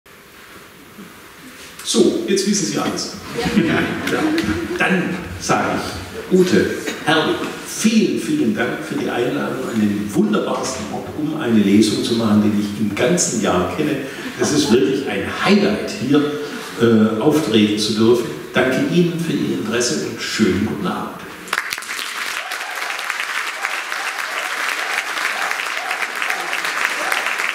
Denis Scheck über seine Lesung bei den Möbelmachern
Alles zur grandiosen Lesung von Denis Scheck anlässlich der 22. Unterkrumbacher Werkstatt Tage ist hier nachzulesen